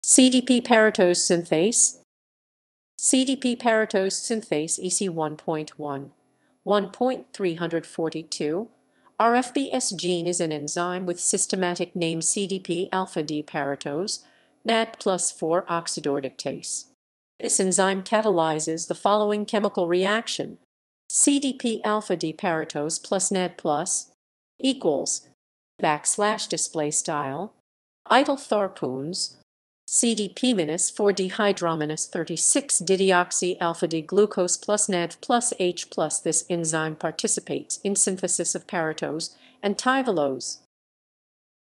CDP-paratose synthase
CDP-paratose synthase (EC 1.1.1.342, rfbS (gene)) is an enzyme with systematic name CDP-alpha-D-paratose:NADP+ 4-oxidoreductase. This enzyme catalyses the following chemical reaction

CDP-alpha-D-paratose + NADP+

    {\displaystyle \rightleftharpoons }
   CDP-4-dehydro-3,6-dideoxy-alpha-D-glucose + NADPH + H+This enzyme participates in synthesis of paratose and tyvelose.